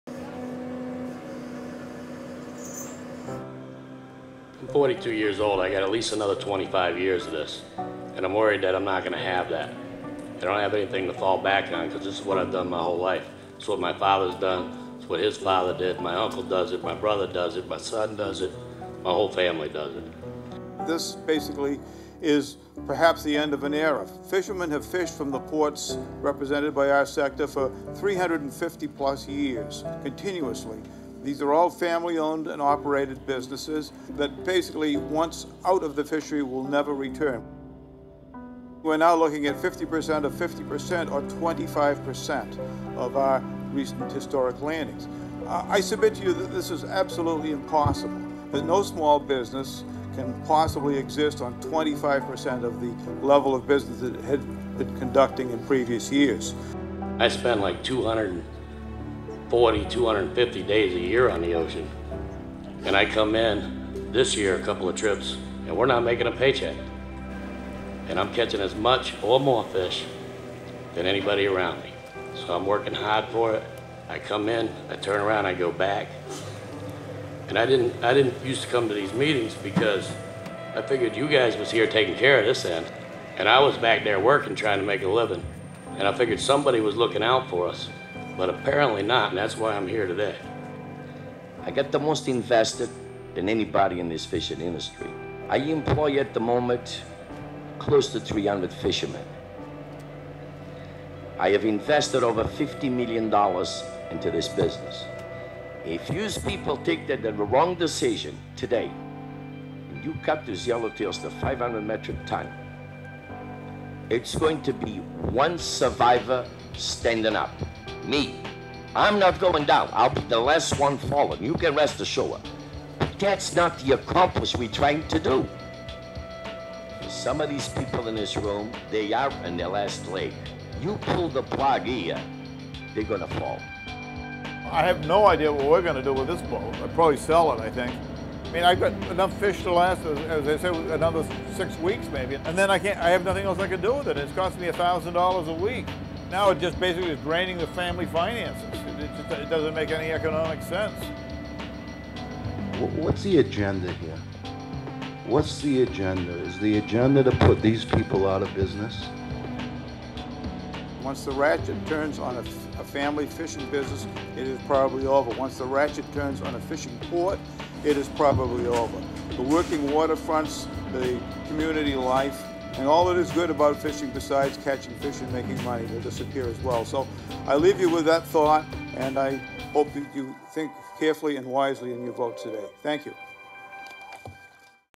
Fish Teaser
Teaser for a documentary about the New England groundfish industry.